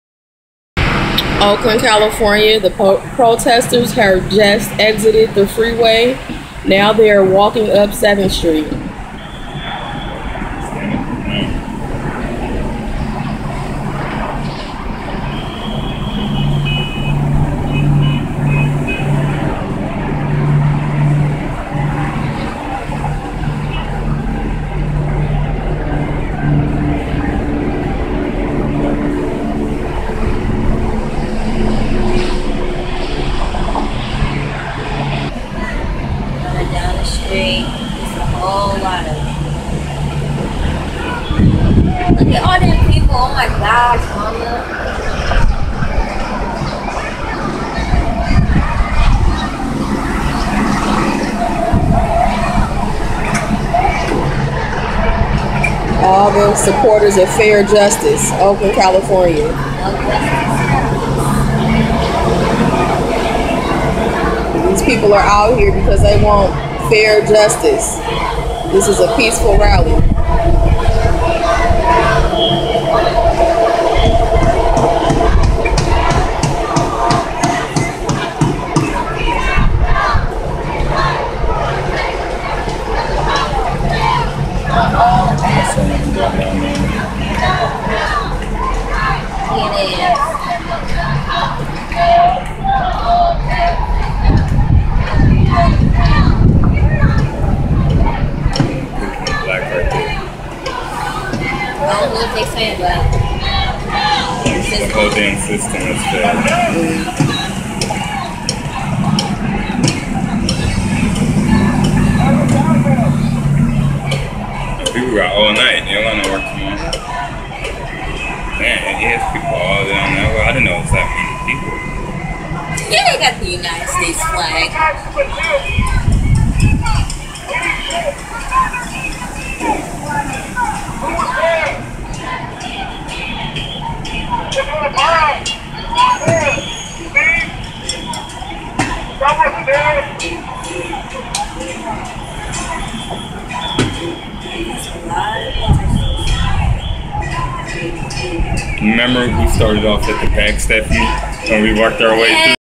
Peaceful protesters Oakland California
Protesters block freeway and  Oakland California